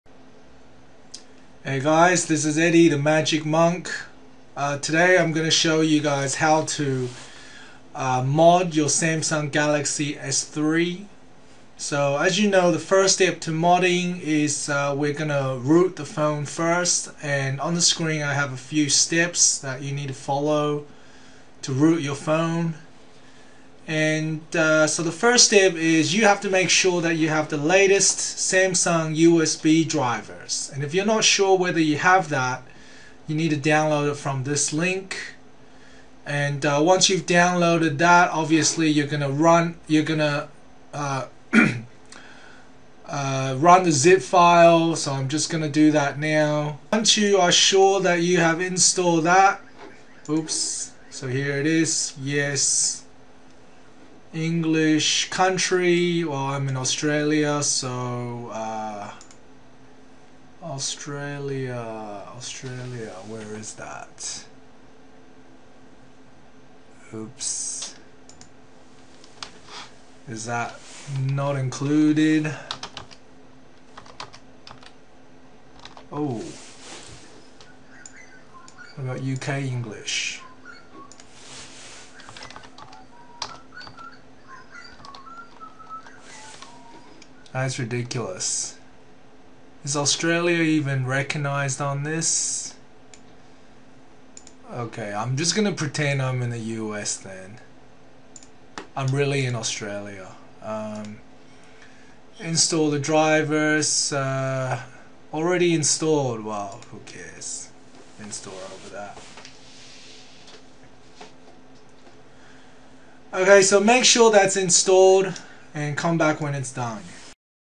Samsung Galaxy S3 unlocking / rooting / modding tutorial part 1: Installing Samsung USB Drivers
*This video only documents my personal experience - I cannot guarantee that the same process will work for your phone and take no responsibility for whatever damage that could be caused to your phone as a result of following this tutorial.

How to unlock / root / mod Samsung Galaxy S3